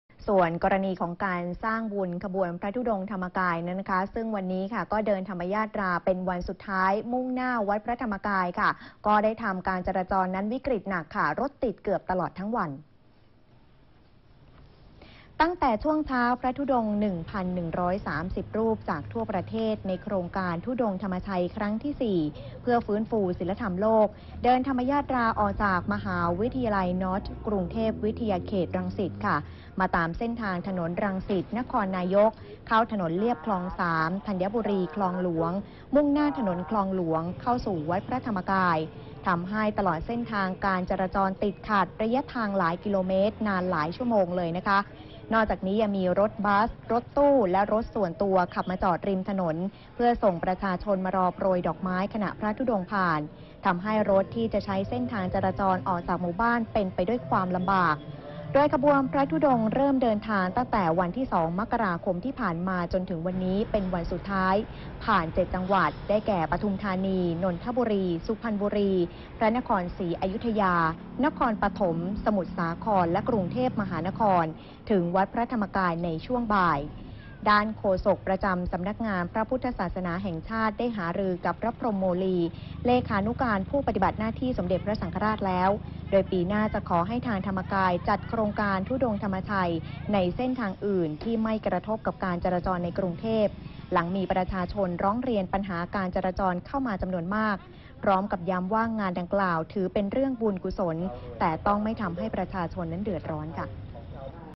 วัดพระธรรมกายจัดธุดงค์ ทำจราจรติดขัดหนัก
ตั้งแต่ช่วงเช้าพระธุดงค์ 1,130 รูปจากทั่วประเทศ ในโครงการธุดงค์ธรรมชัยครั้งที่ 4 เพื่อฟื้นฟูศีลธรรมโลก เดินธรรมยาตราออกจากมหาวิทยาลัยนอร์ทกรุงเทพ วิทยาเขตรังสิต มาตามเส้นทางถนนรังสิต-นครนายก เข้าถนนเลียบคลองสาม ธัญบุรี-คลองหลวง มุ่งหน้าถนนคลองหลวง เข้าสู่วัดพระธรรมกาย ทำให้ตลอดเส้นทางการจราจรติดขัด ระยะทางหลายกิโลเมตรนานหลายชั่วโมง

นอกจากนี้ยังมีรถบัส รถตู้ และรถส่วนตัวขับมาจอดริมถนน เพื่อส่งประชาชนให้ลงมานั่งรอโปรยดอกไม้ขณะพระธุดงค์เดินผ่าน ทำให้รถที่จะใช้เส้นทางจราจรออกมาจากหมู่บ้านเป็นไปด้วยความลำบาก

โดยขบวนพระธุดงค์เริ่มเดินทางตั้งแต่วันที่ 2 มกราคมที่ผ่านมา จนถึงวันนี้เป็นวันสุดท้าย ผ่าน 7 จังหวัด ได้แก่ ปทุมธานี นนทบุรี สุพรรณบุรี พระนครศรีอยุธยา นครปฐม สมุทรสาคร และกรุงเทพมหานคร ถึงวัดพระธรรมกายในช่วงบ่าย